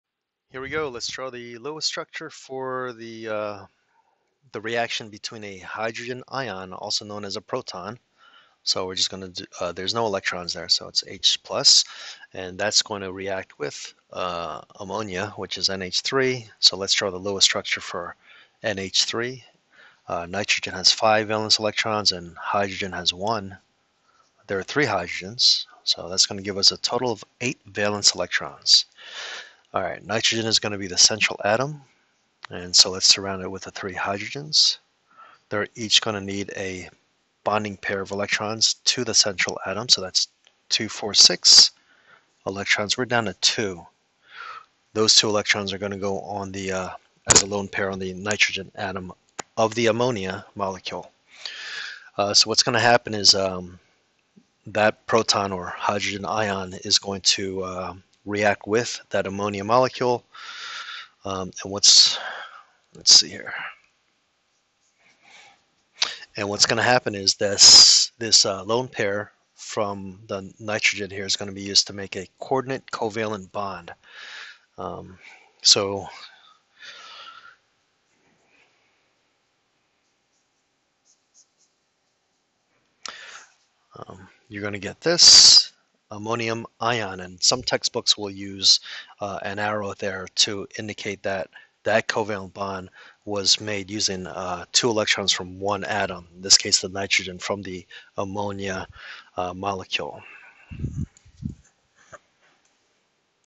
Lewis Structure of H+ and Ammonia (NH3) to form Ammonium Ion NH4+ with Coordinate Covalent Bond
Draw the Lewis Structure for the Reaction between a Hydrogen Ion / Proton [H]+ and Ammonia NH3 to form the Ammonium Cation [NH4]+ using a Coordinate Covalent Bond